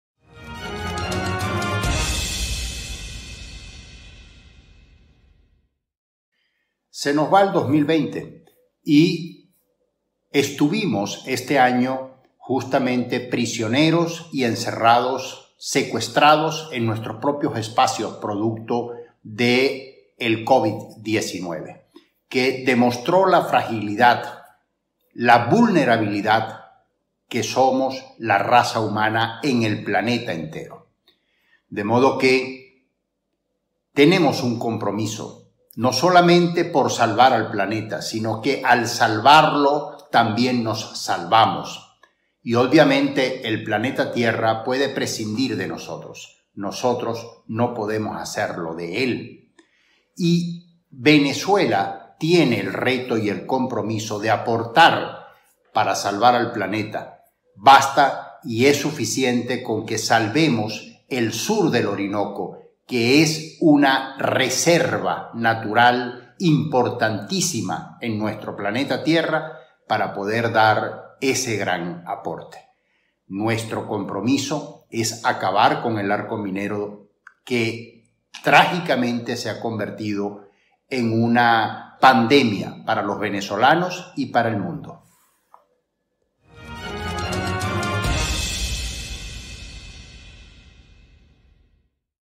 EL MEJOR APORTE DE VZLA POR SALVAR AL PLANETA 28Dic Américo de Grazia
El mejor aporte de Vzla por salvar al planeta, es acabar con el ArcoMinero y sus efectos destructivos y holocausticos